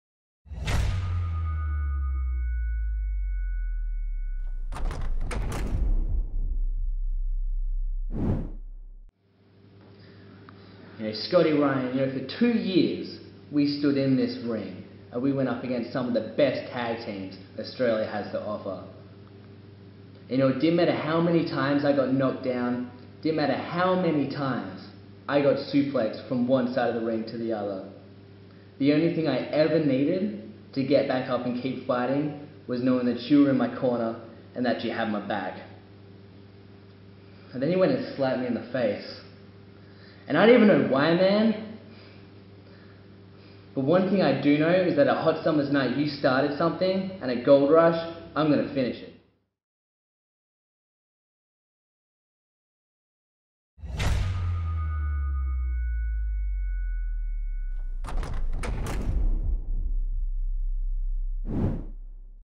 Leo Pratt has a message for his fellow Dropkick
At Hot Summer's Night an all out brawl occurred between the two Dropkicks, while Leo Pratt doesn't know why the implosion occurred he's ready for Goldrush